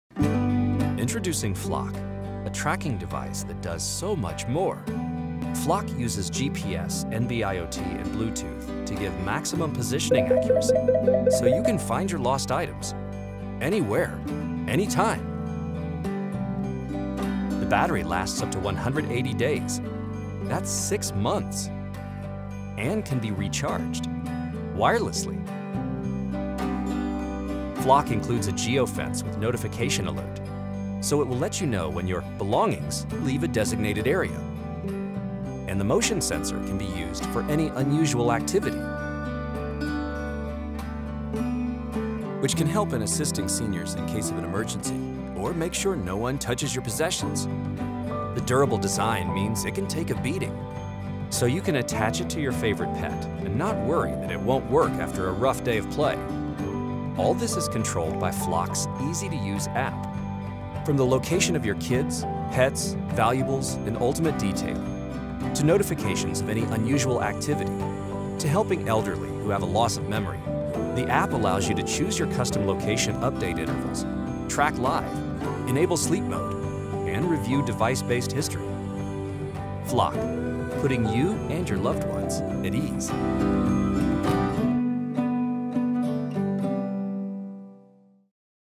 Flok: The most reliable next-generation GPS tracker
Flok has an unlimited working radius, up to 180 days of battery life & advanced GPS tracking to locate loved ones, pets and valuables. Flok will launch Monday 11/11 on Kickstarter.